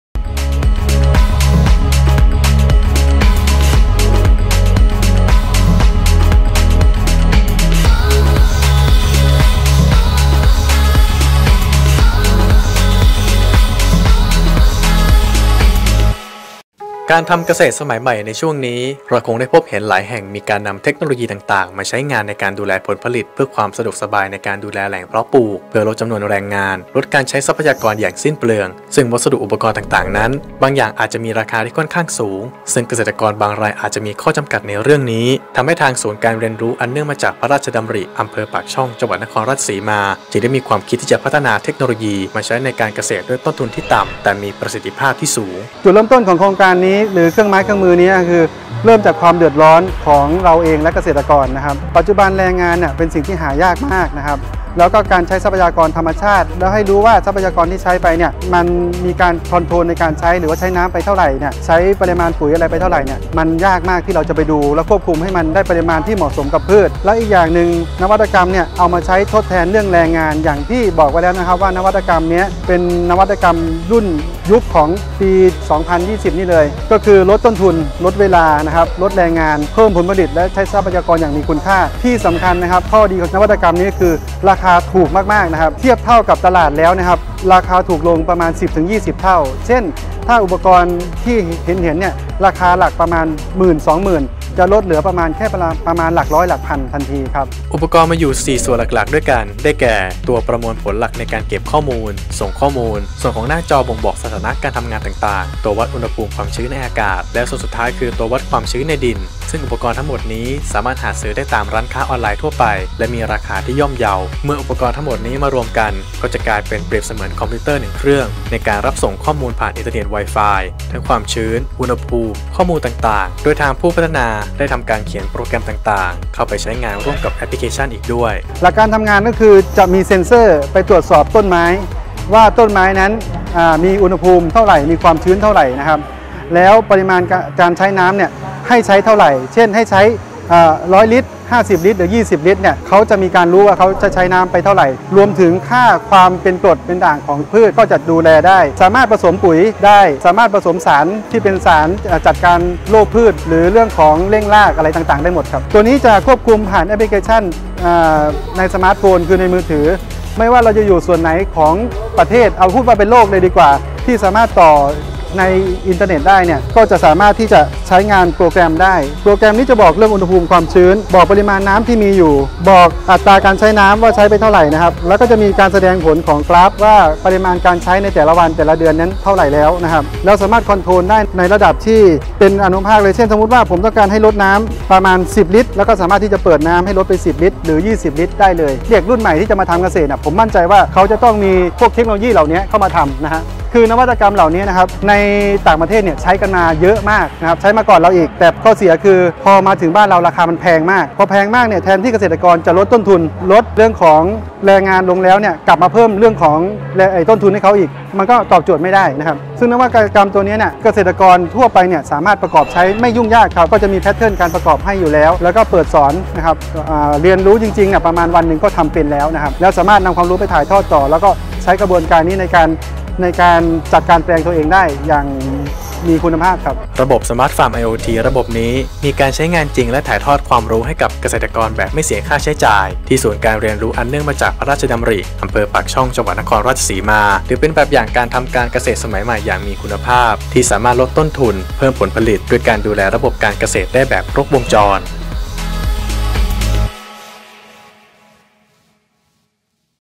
ระบบ Smart farm IOT ลดต้นทุนเกษตรกร
ระบบ Smart farm IOT ลดต้นทุนเกษตรกร : ผลงานจากศูนย์การเรียนรู้อันเนื่องมาจากพระราชดำริ จ.นครราชสีมา เป็นนวัตกรรมที่ช่วยลดการใช้แรงงาน ลดเวลา และลดต้นทุนในการทำแปลงเกษตร ช่วยให้ใช้ทรัพยากรอย่างคุ่มค่า

ขอขอบคุณ Clip Video : MOST Science Channel
ติดตามข่าวสารเพิ่มเติมได้ทาง